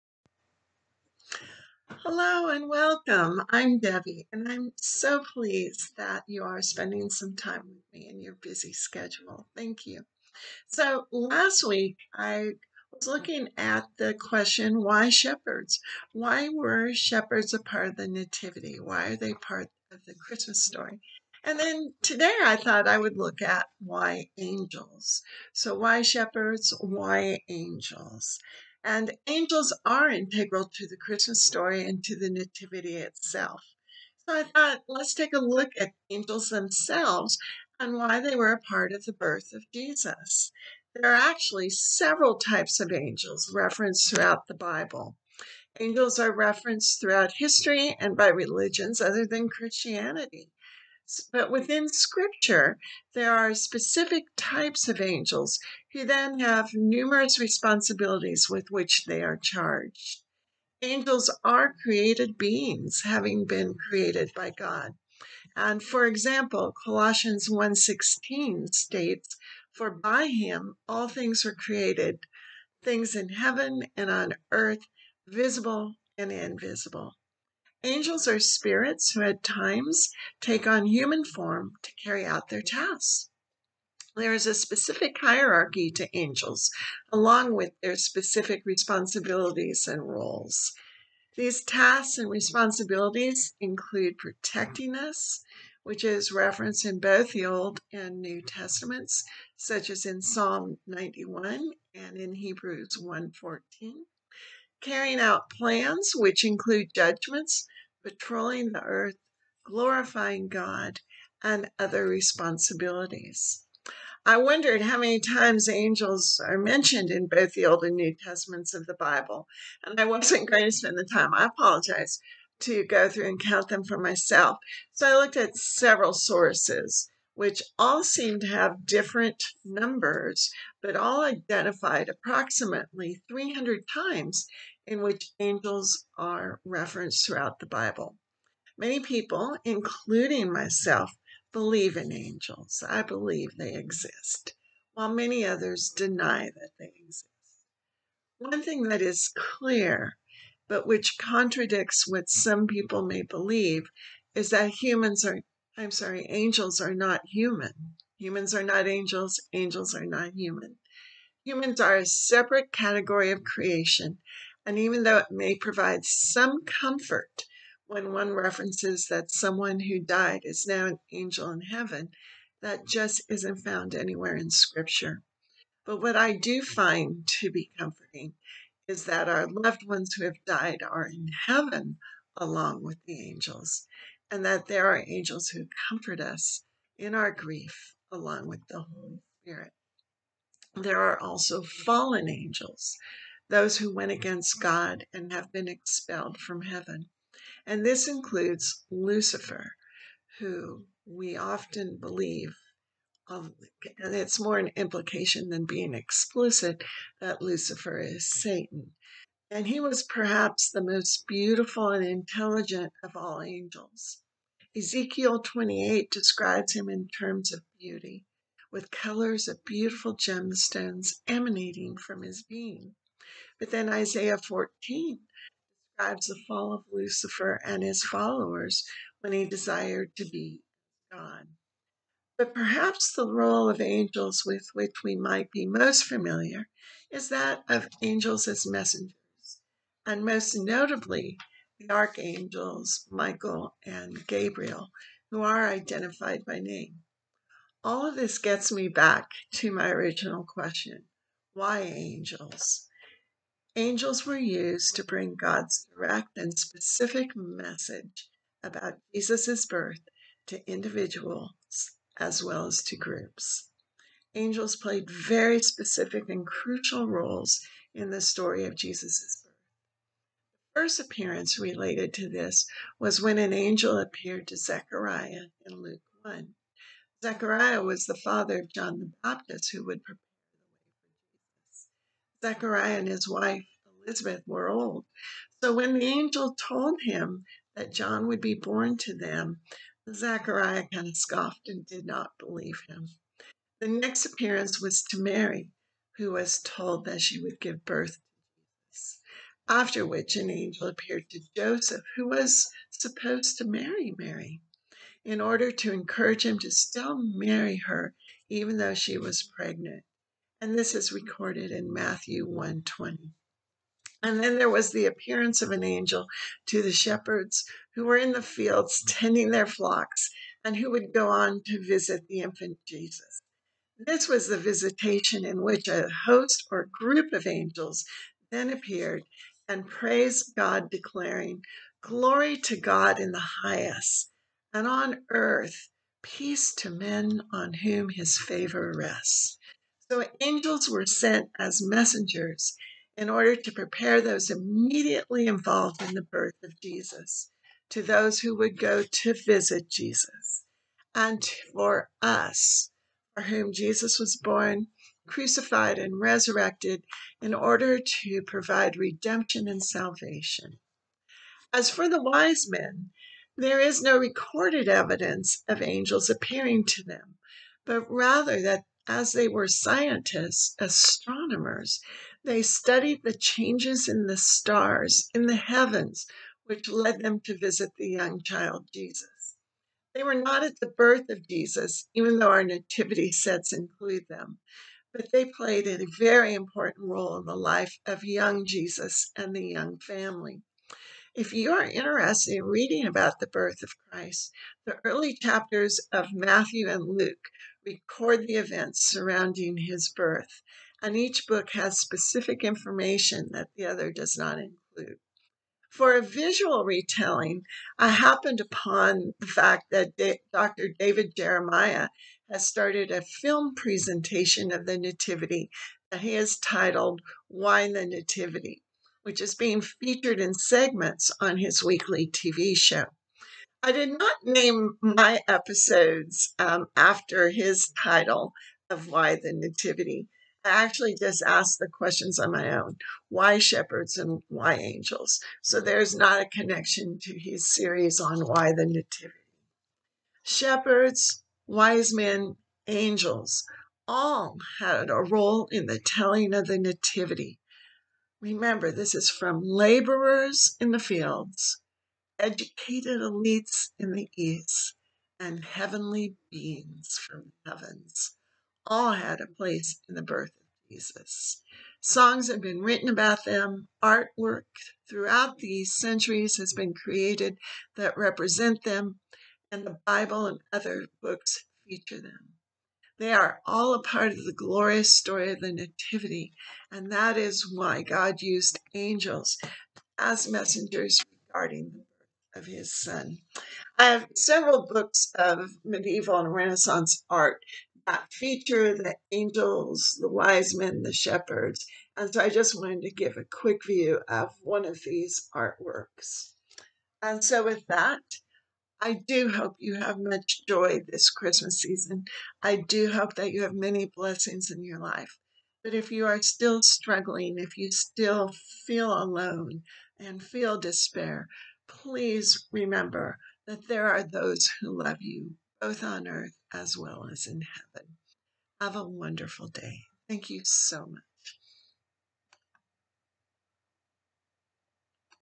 Christmas: Why Angels?
A discussion as to why angels, along with shepherds and wise men, are an essential element of the Christmas story.